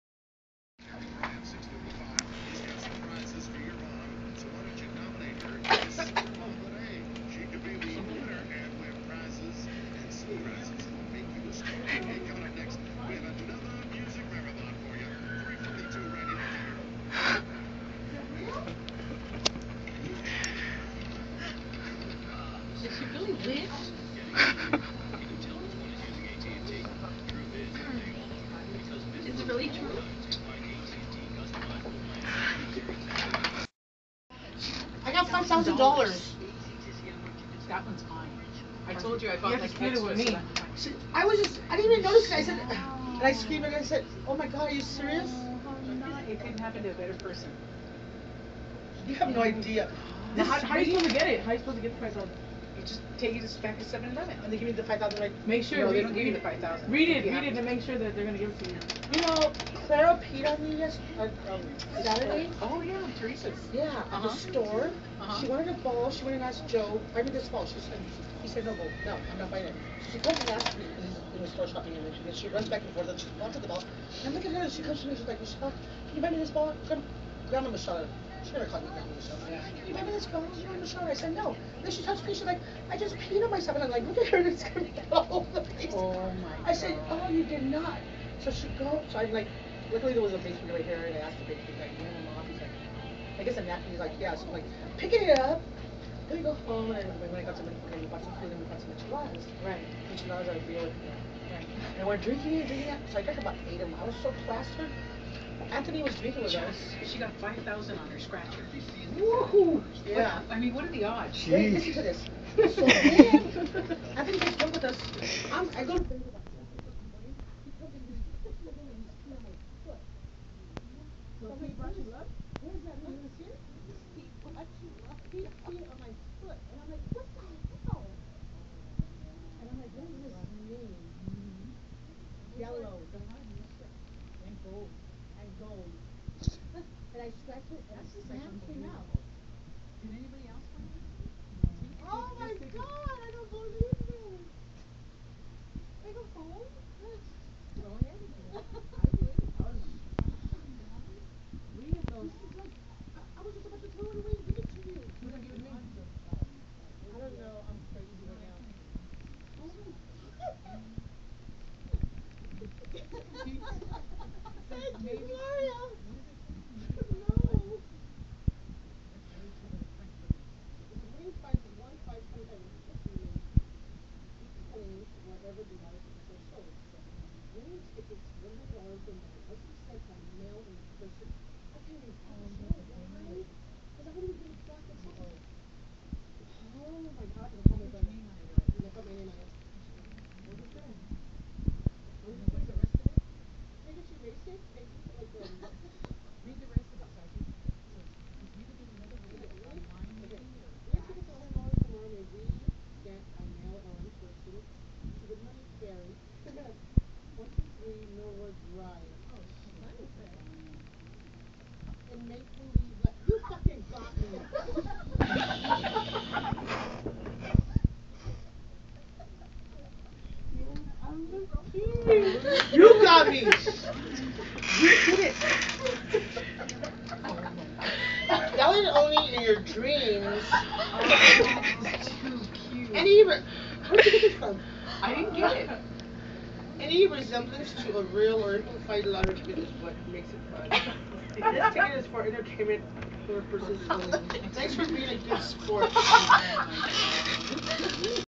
April Fools Lotto Scratcher Prank AllstateLending
From my android : Coworker gets Pranked takes her so long to realize.. Lol..she was a good sport